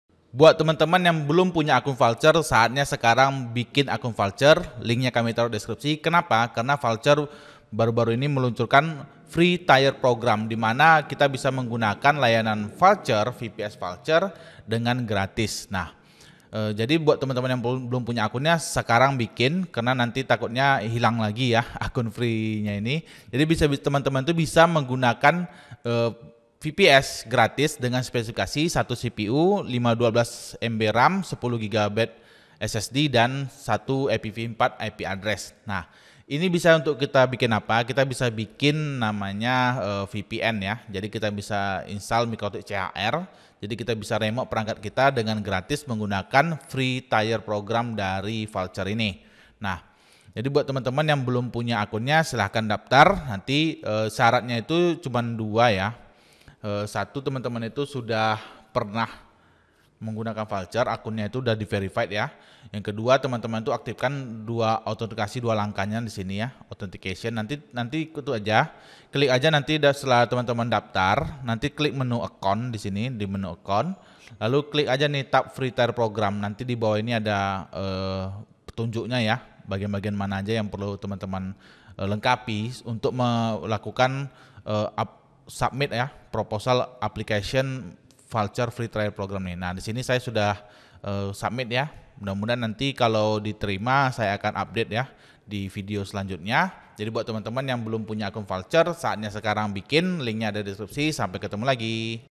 VPS Gratis dari VULTR, Buruan Daftar Sekarang
Download Ebook 101 Benchmark VPS
🔥Eksklusif untuk Anda! Tutorial Premium yang hanya kami share via E-mail

di video ini kami membagikan informasi vps gratis dari vultr yang bisa teman-teman dapatkan dengan cara mendaftar free tier program dari vultr, buat teman-teman yang belum punya akun vultr silahkan daftar di link ini

Apa itu Free Tier Program VULTR?
Free Tier Program di Vultr adalah program yang menawarkan layanan gratis bagi pengguna untuk mengakses server virtual selama periode tertentu. Program ini memungkinkan pengguna untuk mendapatkan akses ke CPU, RAM, dan ruang penyimpanan tertentu tanpa biaya.

Dalam Free Tier Program, Vultr menawarkan akses gratis ke beberapa server virtual yang dapat dipilih oleh pengguna. Server ini dilengkapi dengan sistem operasi Linux, termasuk CentOS, Ubuntu, Debian, Fedora, dan lainnya.

Namun, meskipun program ini gratis, ada beberapa batasan yang perlu diperhatikan. Misalnya, pengguna hanya dapat mengakses satu server virtual gratis per akun, dan terdapat batasan pada jumlah CPU, RAM, dan penyimpanan yang tersedia. Selain itu, layanan ini hanya tersedia untuk pengguna baru dan terbatas pada periode tertentu.

Meskipun terdapat batasan pada layanan gratis yang tersedia, Free Tier Program di Vultr dapat menjadi pilihan yang menarik bagi pengguna yang ingin mencoba layanan server virtual tanpa harus membayar biaya awal yang tinggi.

apa itu vps gratis?
VPS gratis adalah layanan hosting yang menawarkan akses virtual private server (VPS) secara gratis kepada pengguna. Layanan ini biasanya diberikan oleh penyedia hosting web atau cloud, dan memungkinkan pengguna untuk mengakses sumber daya server virtual tertentu tanpa harus membayar biaya.